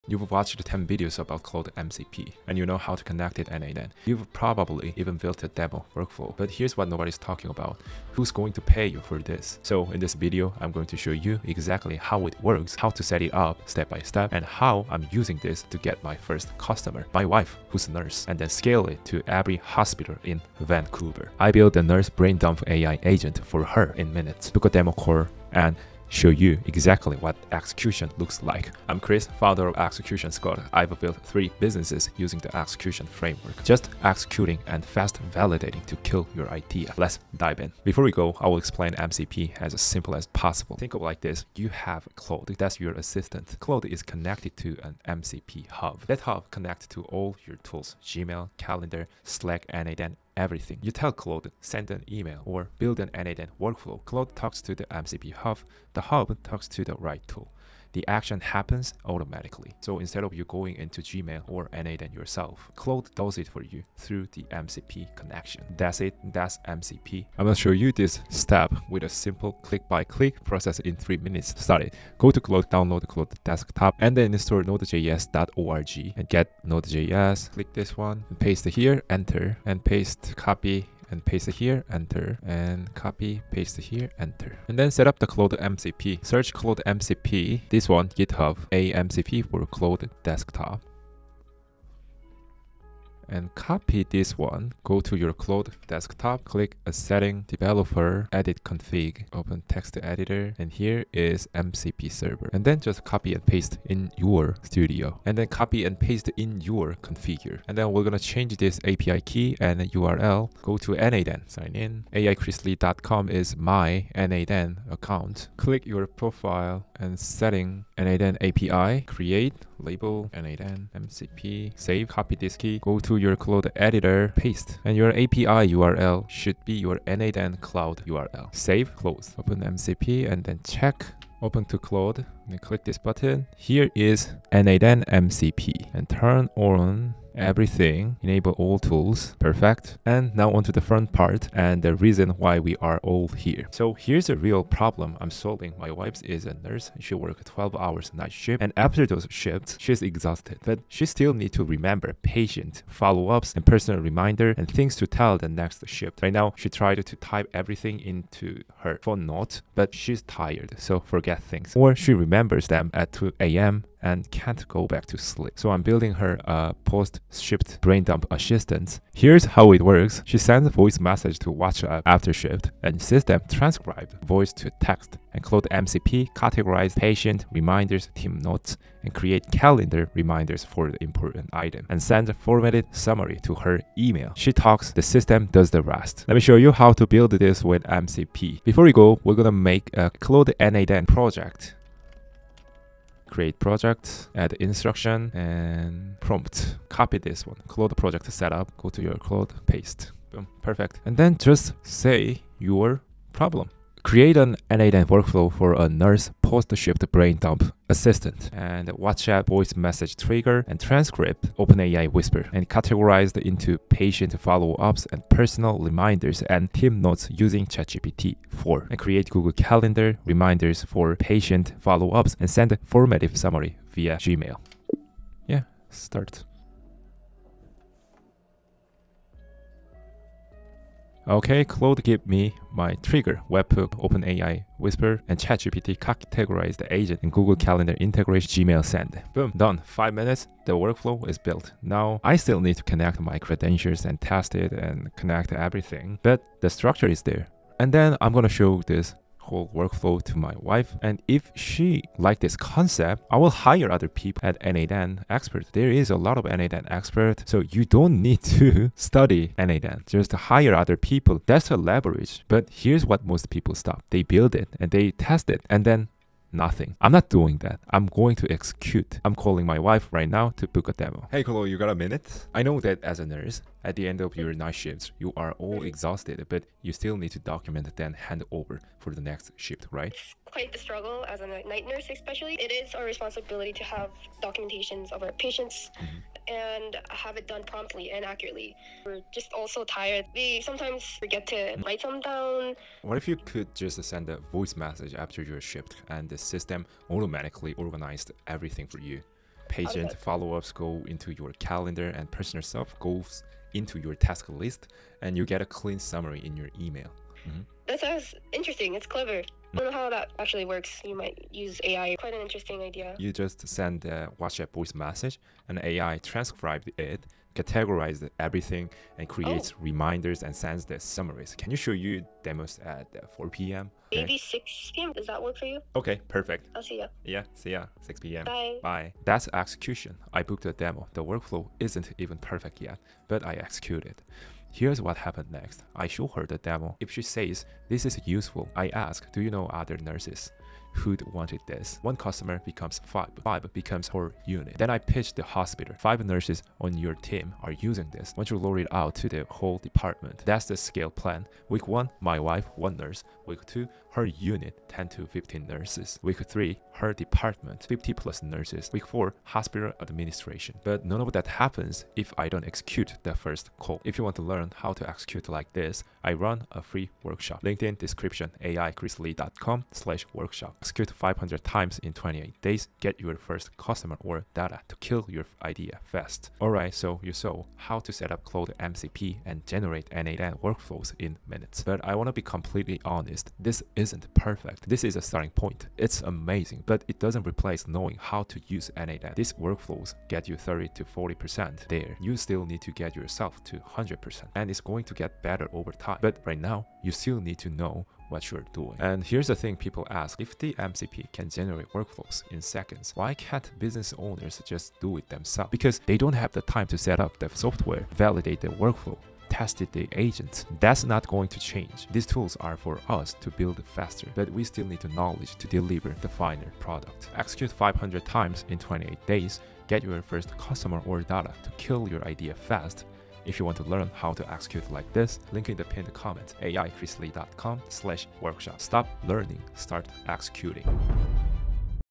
I Sell n8n MCP Automation to My Wife (A Nurse) - Real Demo Call
. * Everyone’s talking about Claude MCP and n8n automation, but nobody’s asking the real question:
Who’s actually paying for it?

In this video, I’ll show you how to go from demo to dollars.
You’ll learn how to:

Set up Claude MCP stepbystep (no coding fluff).

Connect it to n8n and build a workflow in minutes.

Create a real AI Agent use case, a nurse “brain dump assistant” that organizes notes, reminders, and followups automatically.

Watch me book my first user (my wife, a nurse) live, then plan how to scale it to her entire hospital.

This isn’t another tutorial, it’s execution in action.
You’ll see exactly how to turn AI tools into realworld solutions people actually pay for.

If you’ve been learning Claude, n8n, or AI automation but still have $0 clients, this video will flip your perspective.
It’s not about building faster. It’s about executing smarter.

Stop learning for months. Start validating in days.
Let’s build the future,  one real workflow at a time.

Timestamps:
0:36 MCP Explanation
1:06 StepbyStep Setup
2:19 Building the Workflow
4:28 The Execution Part
6:16 Honest Assessment
6:38 Why Business Owners Won't Do This